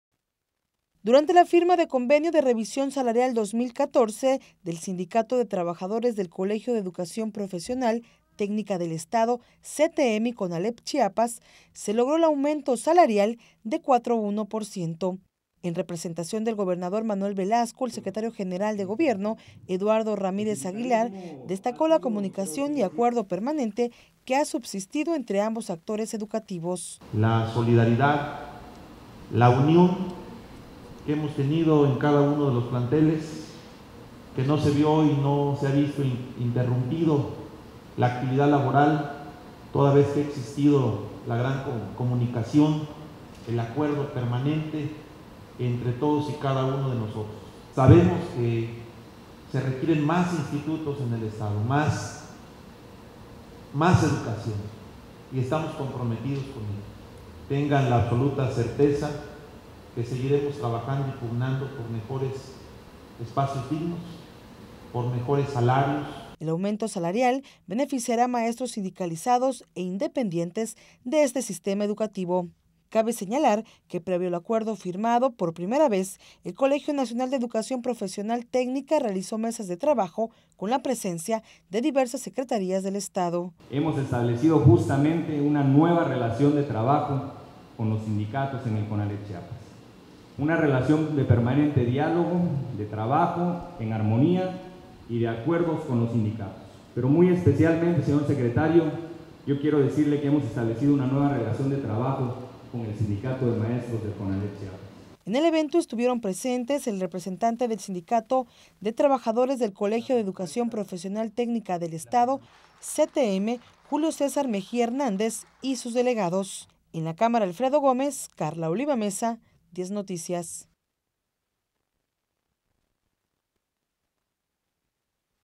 Aumento del 4.1% salarial para los trabajadores del CONALEP-Chiapas
Durante la firma de convenio de "Revisión Salarial 2014", del Sindicato de   Trabajadores del Colegio de Educación Profesional Técnica del estado- CTM y   CONALEP Chiapas, se logró el aumento salarial de 4.1 por ciento.  En representación del gobernador Manuel Velasco, el secretario general   de Gobierno, Eduardo Ramírez Aguilar, destacó la comunicación y acuerdo   permanente que ha subsistido entre ambos actores educativos.